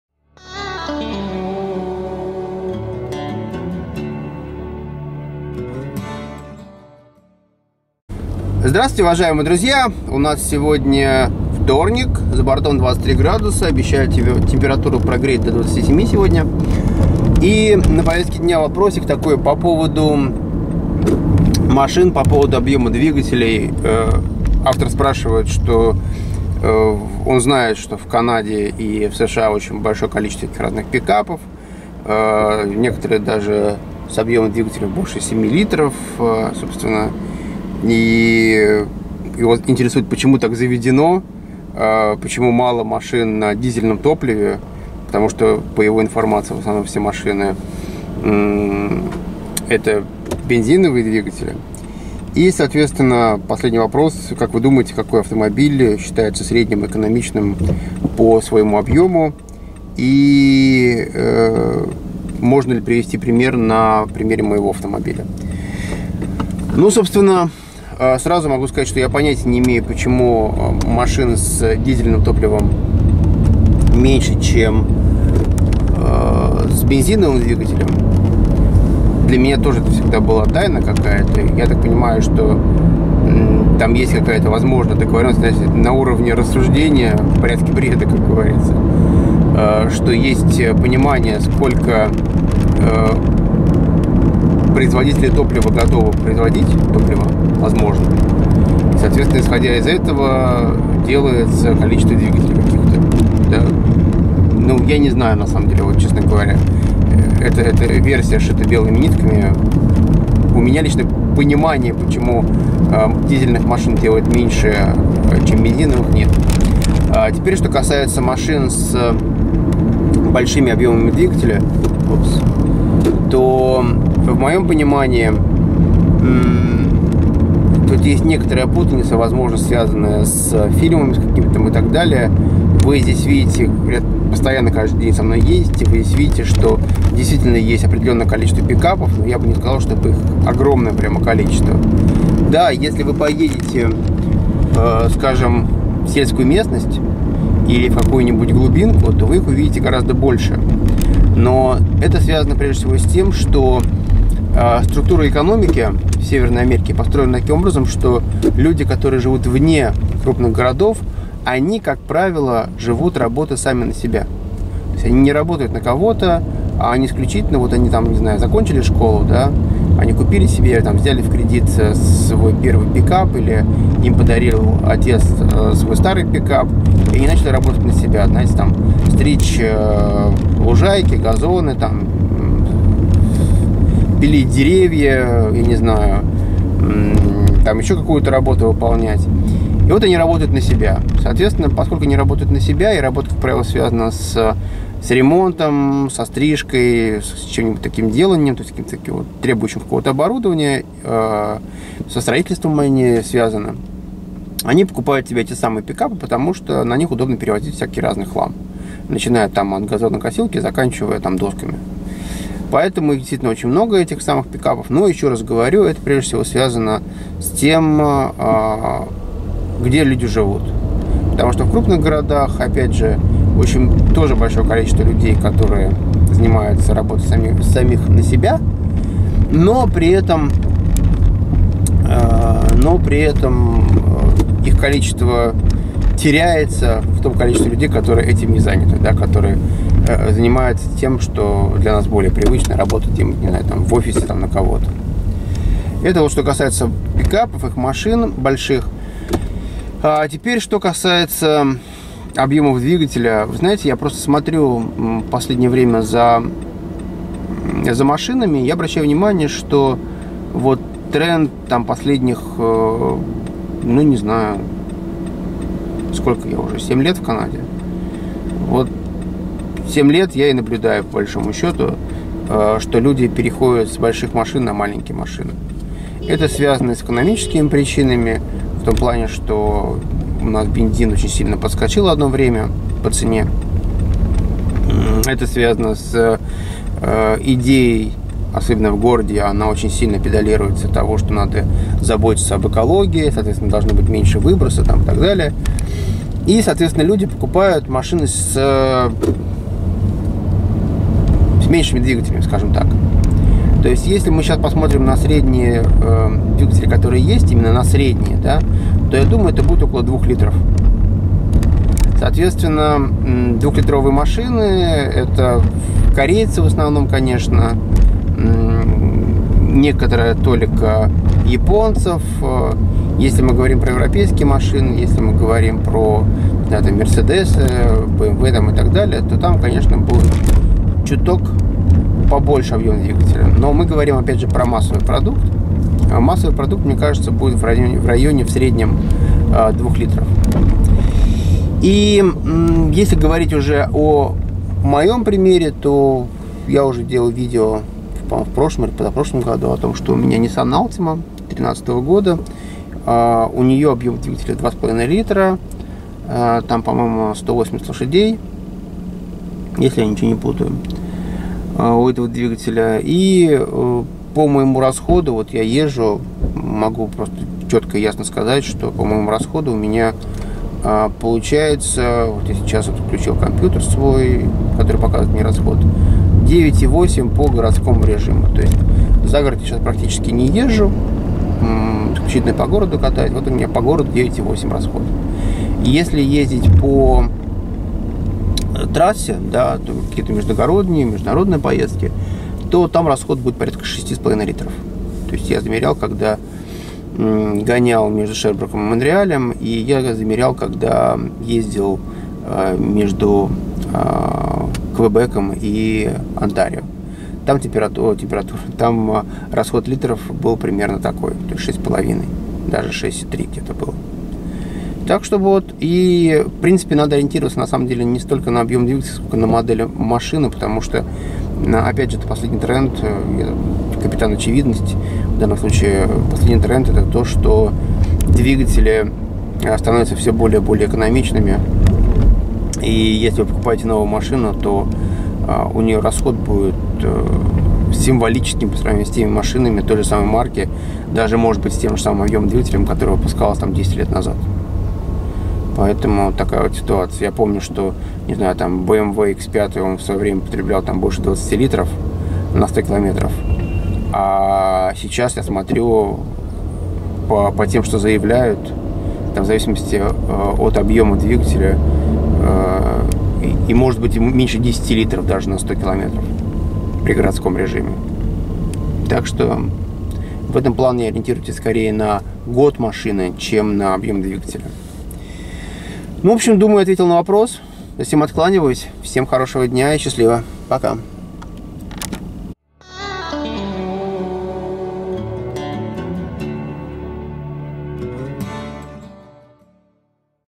Канада 1070: Какой средний объем двигателя у современных машин в Канаде
Отвечаю на вопрос: "Здравствуйте Сергей!
У меня вопрос по поводу северно американского "Автопрома"
Имею ввиду что в северной Америке автомобили с большим объёмом двигателя и ка правило на бензине .
Я сам когда был в США то ездил на пикапах с мотором 7,3 литра!
Интересно почему так заведено и почему так мало автомобилей на дизельном топливе?!
Какое ваше мнение по этому поводу ?
И как Вы думаете автомобиль с каким объёмом считается средним или экономичным в Канаде ? Возможно сможете привести пример на своём авто !?
Заранее благодарю Вас за ответ! " Слушаем, делаем выводы.